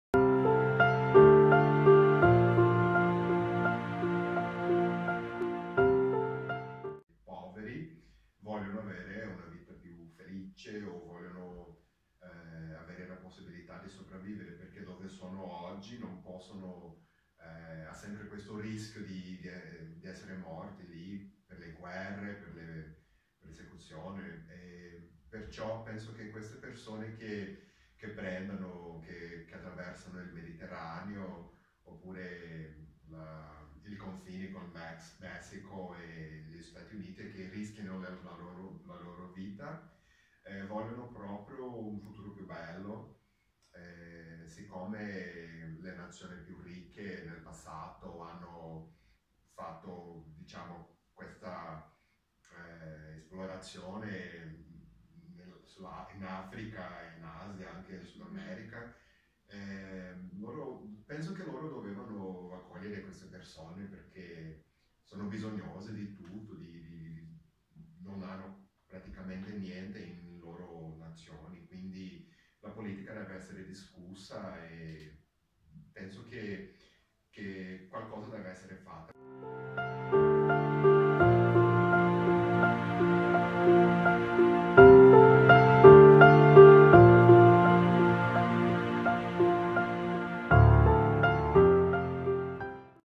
WSA 2016-17 Armi e bagagli. Migrazioni e diritto al futuro - Chi migra non va lasciato solo
Intervista realizzata dalla classe 3C del Liceo Curiel di Padova nel corso del progetto World Social Agenda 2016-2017 di Fondazione Fontana Onlus.
La clip è stata realizzata da Luca Galtarossa del Liceo Cornaro di Padova nel corso della Alternanza Scuola Lavoro.